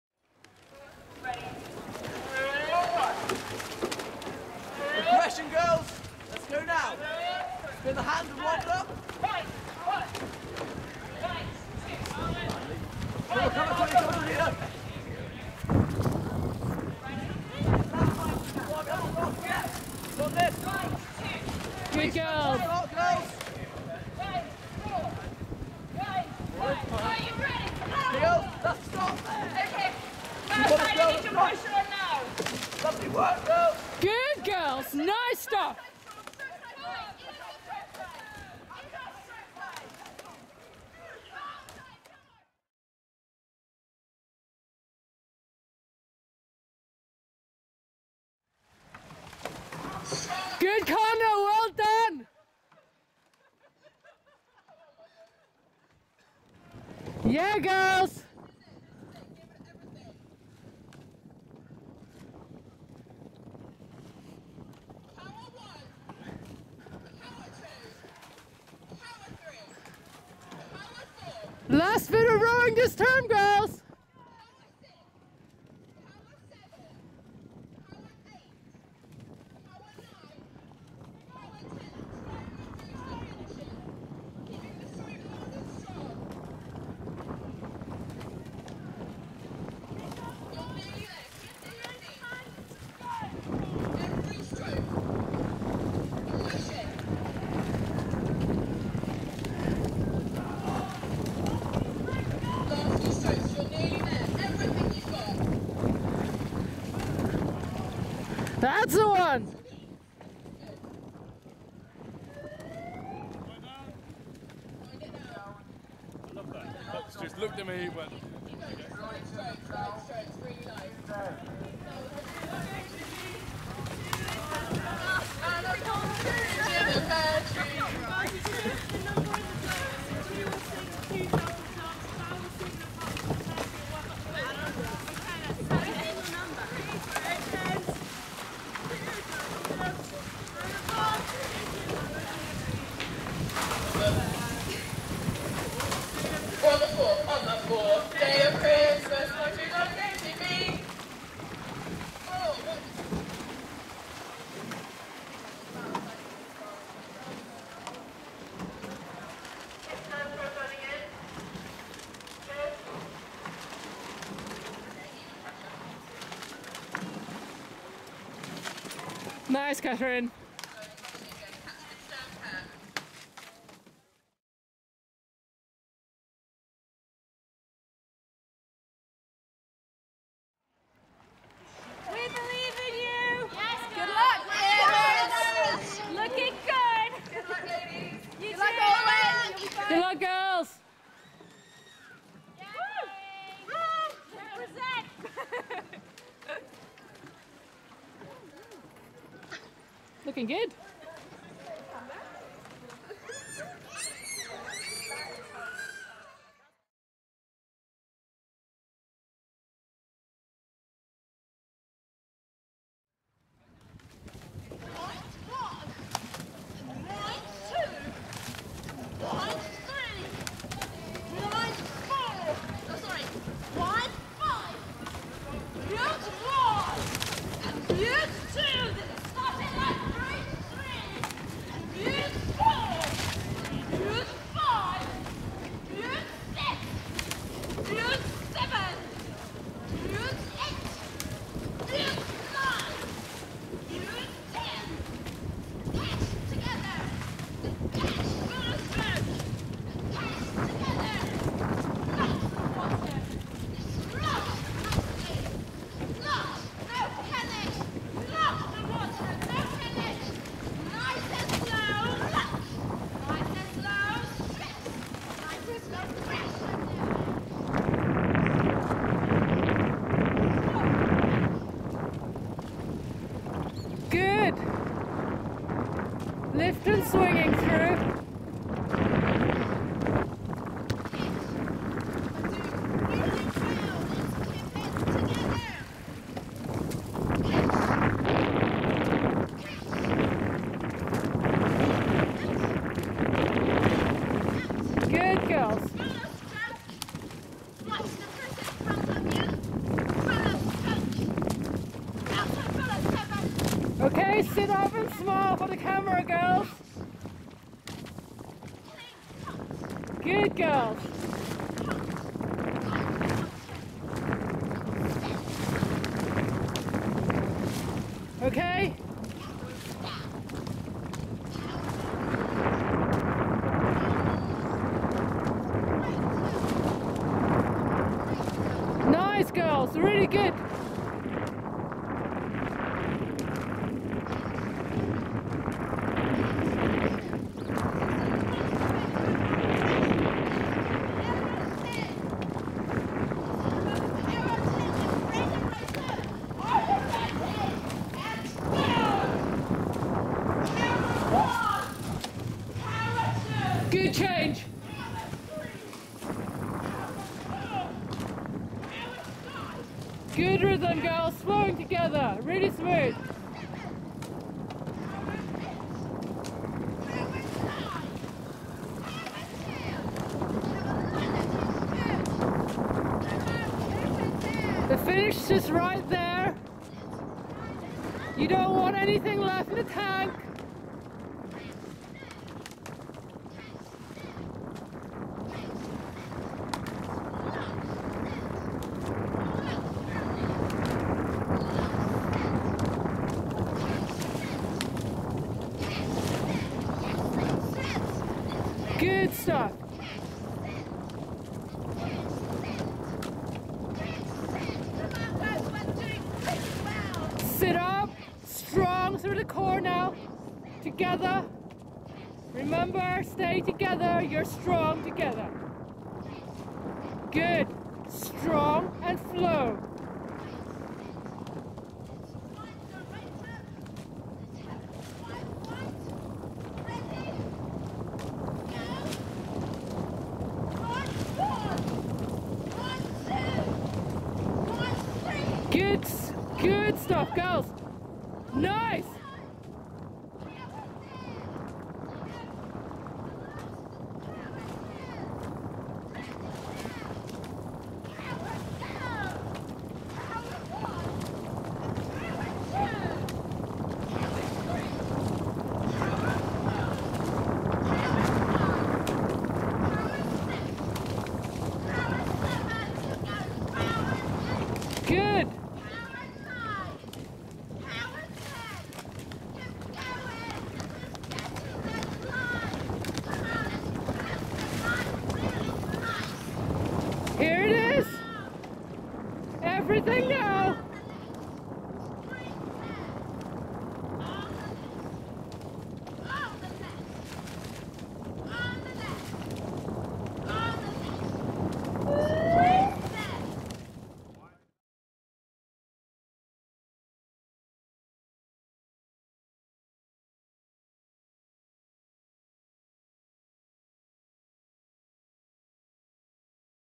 Novice Fairbairns 2014 MECBC all crews
Every crew got at least 30 seconds of footage, though Pina Colada and Tequila Slammers got more than others!